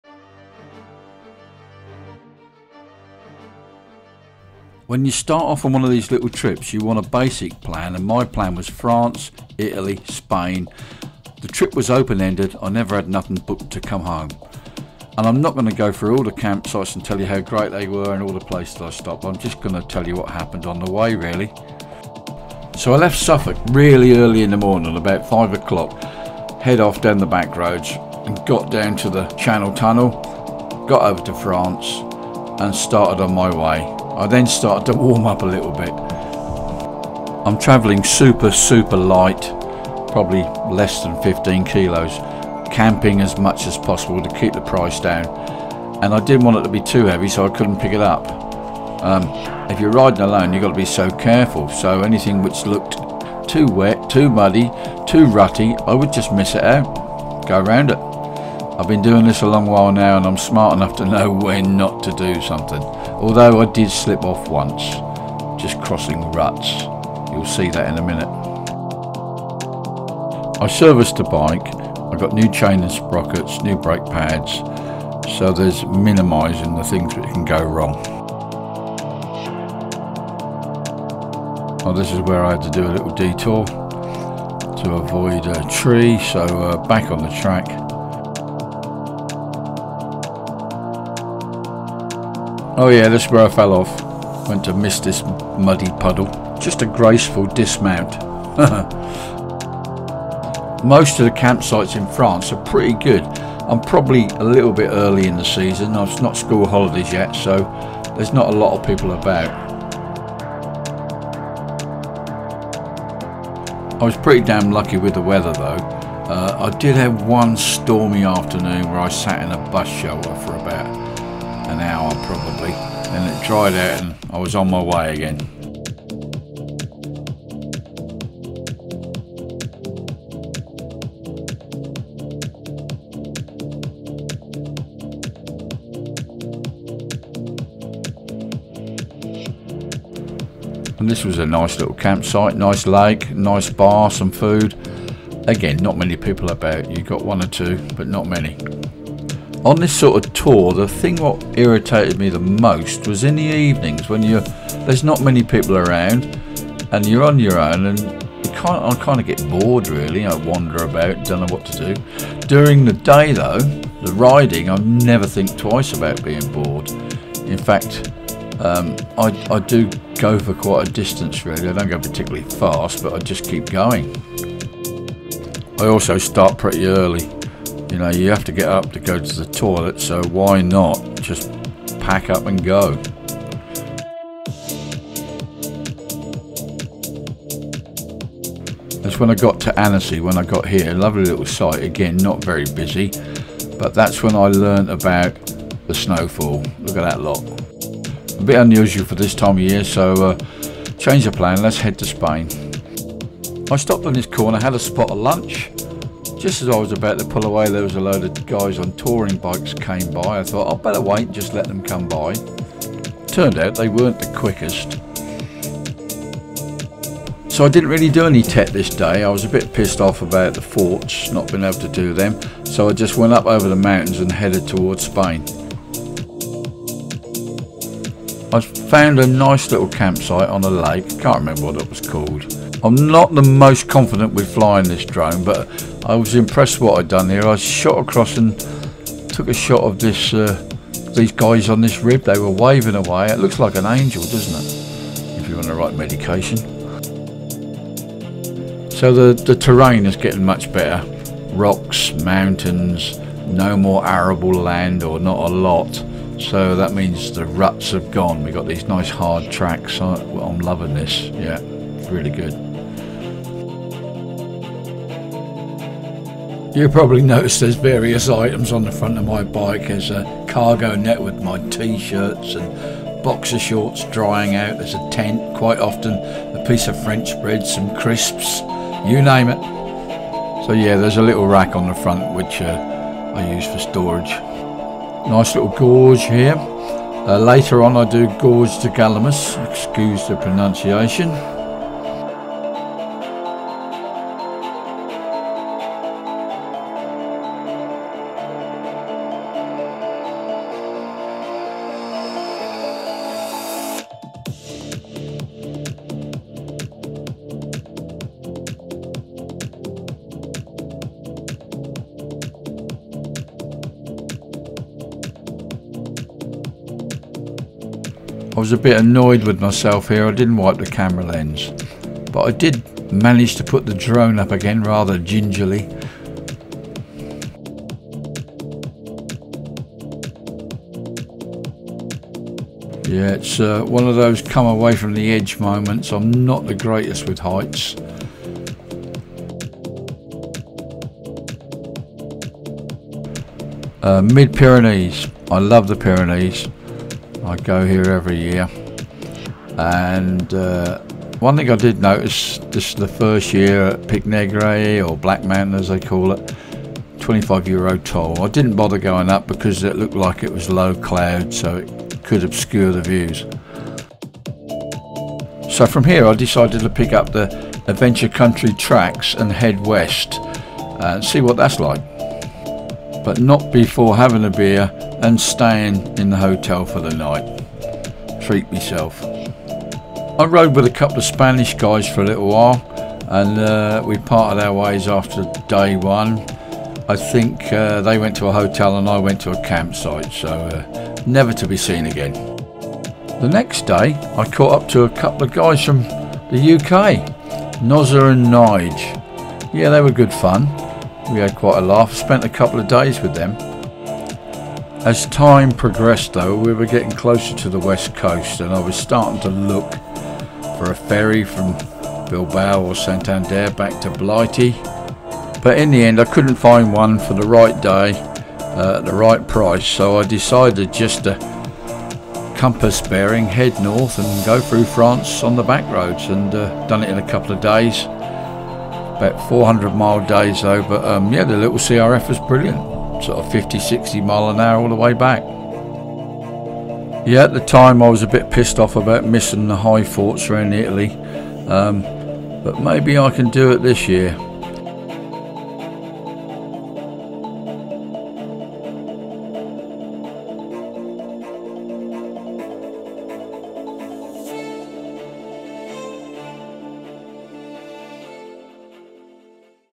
Riding light, euro TET adventure...honda crf 300L
This is my 2023 trip from UK,  my main intention was to visit the WW1 high forts on the Italian TET, well it didn't happen! On the way down some french TET and later across the Pyrenees on the ACT but the Italian TET eluded me with late snowfall putting it out of reach...